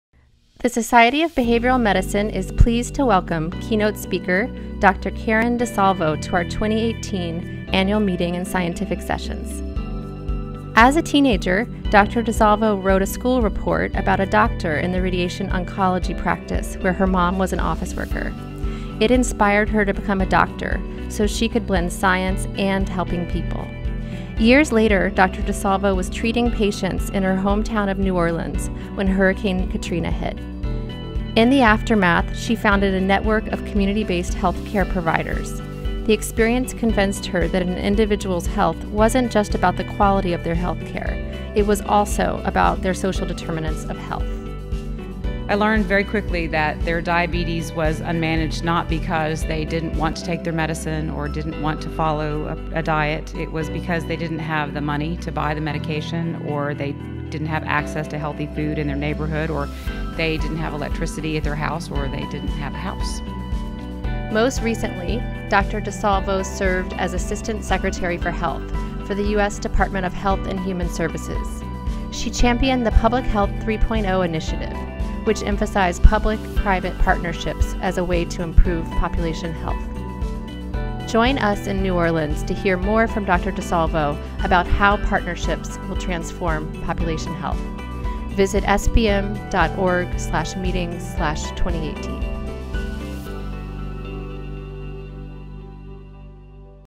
SBM2018 Welcomes Karen DeSalvo
The Society of Behavioral Medicine is pleased to announce Karen DeSalvo, MD, MPH, MSc, will be a keynote speaker at our 2018 Annual Meeting & Scientific Sessions. Dr. DeSalvo served as assistant secretary for health in the U.S. Department of Health and Human Services.